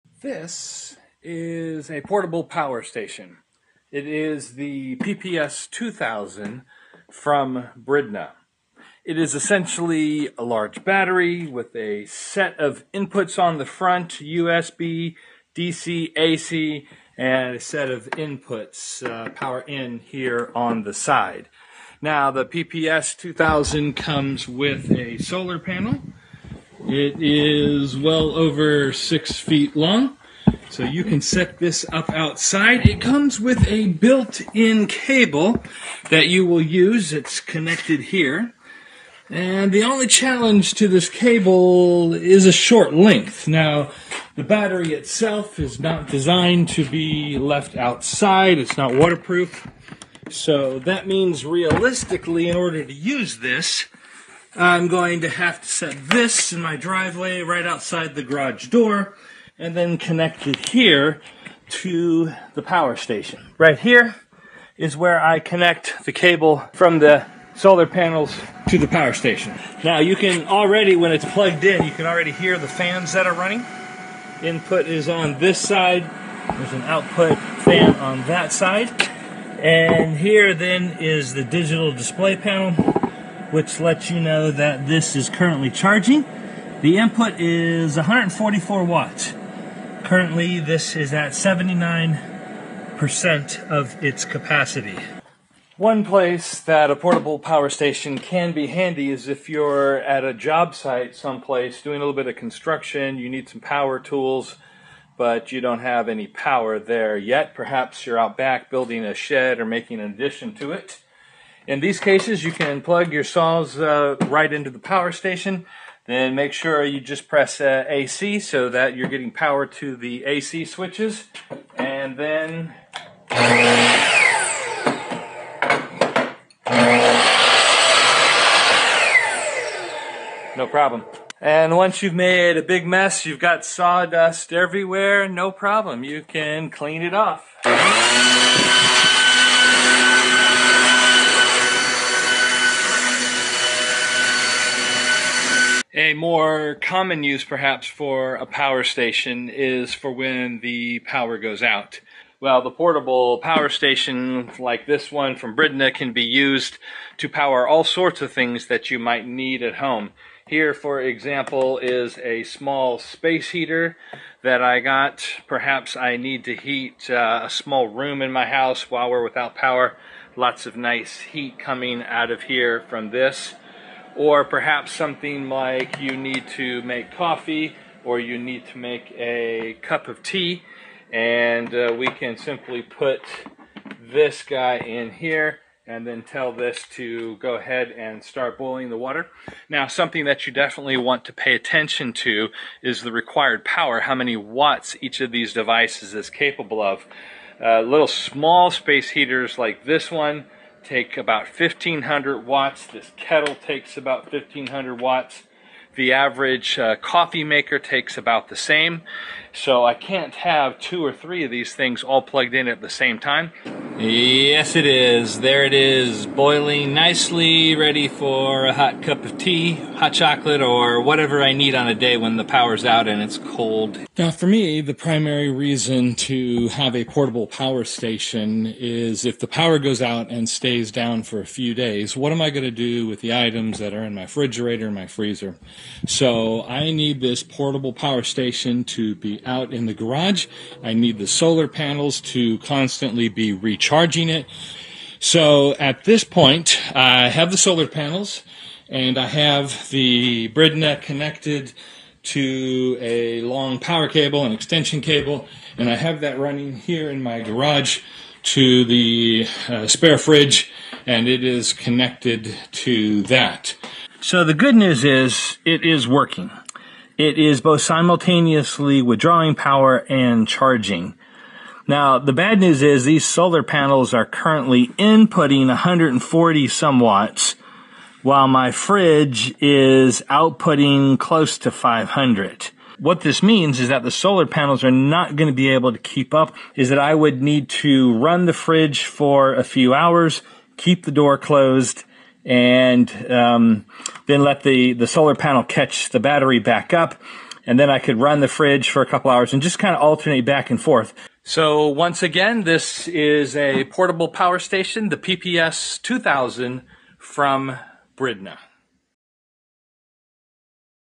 Testing the Bridna PPS2000 Portable Power Station
This video is a part of a review for The Gadgeteer.